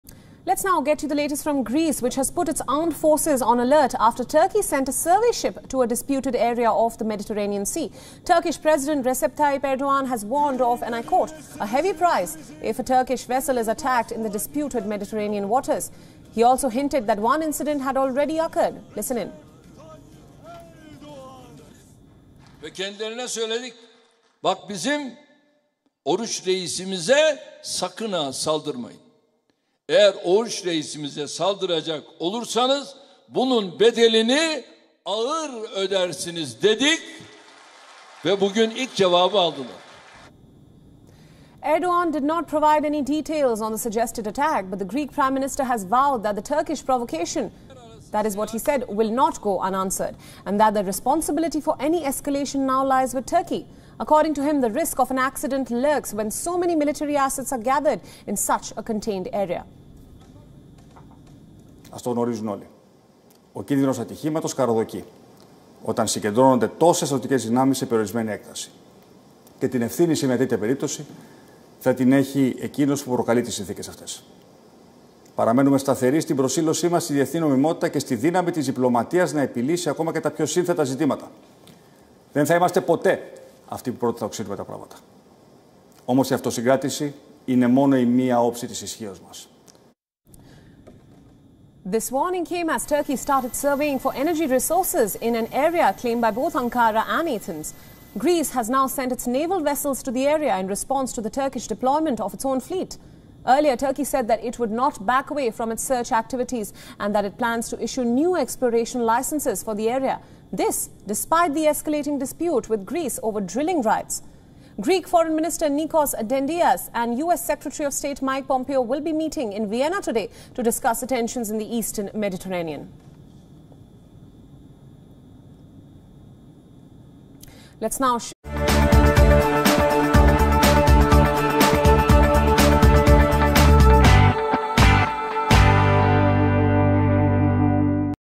Greece-Turkey tensions | 'Turkish provocation won't go unanswered', says Greece | World news
Greece has put its armed forces on alert after Turkey sent a survey ship to a disputed area of the Mediterranean sea. Watch report.